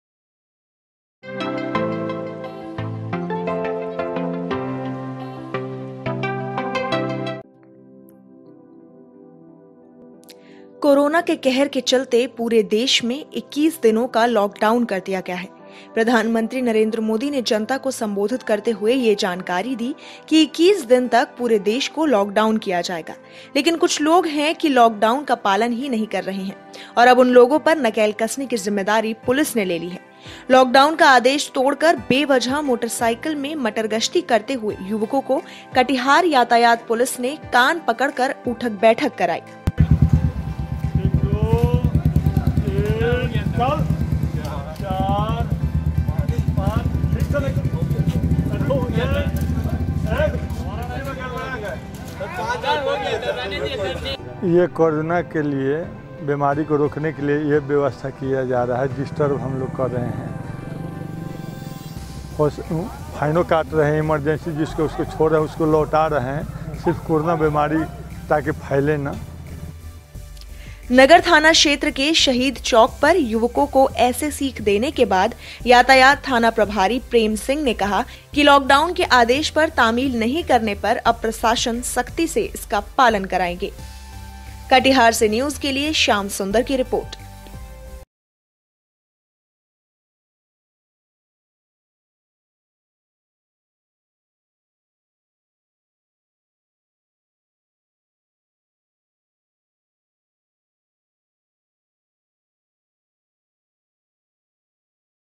Katihar: Locakdown का पालन ना करने वालों को पुलिस ने ऐसे सिखाया सबक
कोरोना के कहर के चलते पूरे देश में 21 दिनों का लॉक डाउन कर दिया गया है प्रधानमंत्री नरेंद्र मोदी ने जनता को संबोधित करते हुए यह जानकारी दी कि 21 दिनों तक पूरे देश को lockdown किया जाएगा लेकिन कुछ लोग है कि लोग डाउन का पालन ही नहीं कर रहे अब उन लोगों पर नकेल कसने की जिम्मेदारी पुलिस ने ले ली है…..